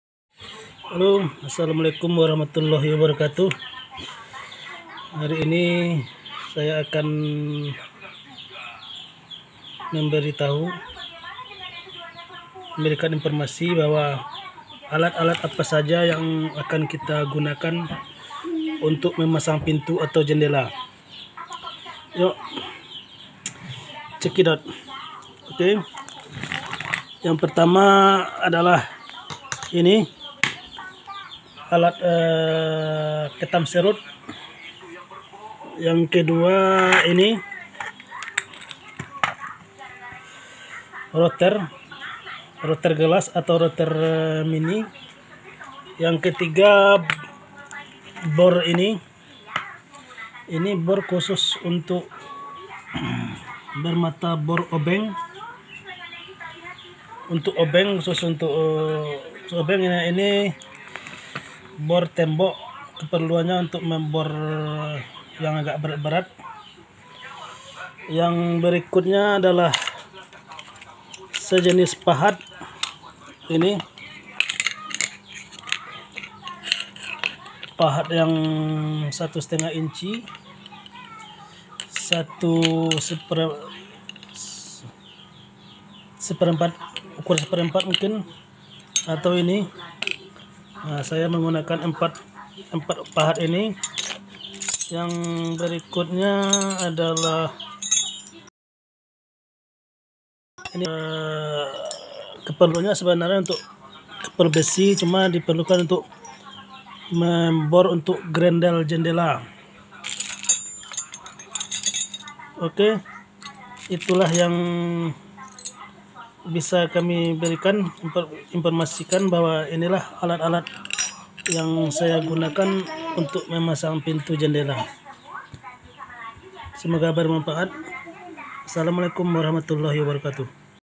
cara cepat memasang pintu ini Alat-alat yang di gunakan untuk tukang kayu.
assalamualaikum ww.
kali saya memberikan tutorial yaitu alat alat apa saja supaya bisa cepat memasang pintu dan jendela.
alat ini membantu kita supaya lebih cepat dalam mengerjakan memasang pintu dan jendela.
tonton ya sampa selesai
wassalam.